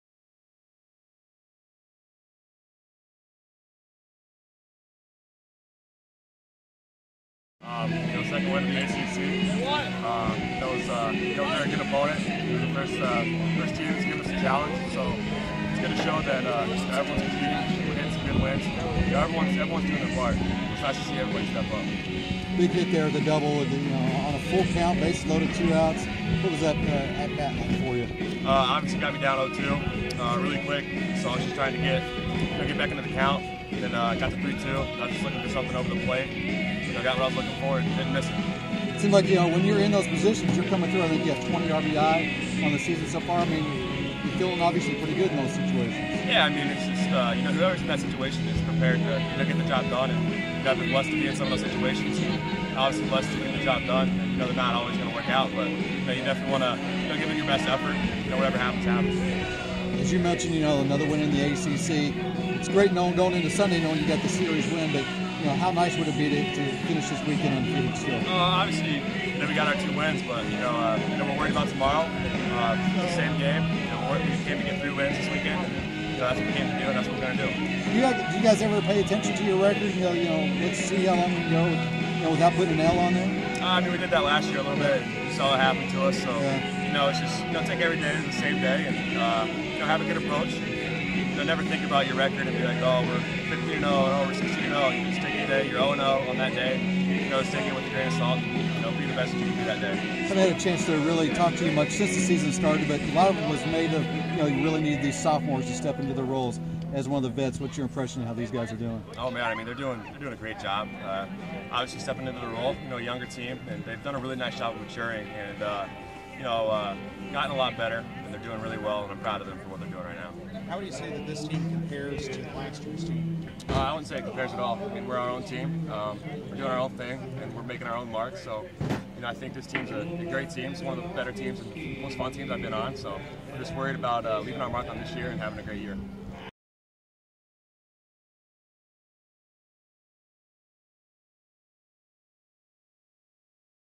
BSB: Devin Mann Press Conference vs. North Carolina
BSB: Junior second baseman Devin Mann discusses Louisville's 6-4 win over North Carolina on March 10.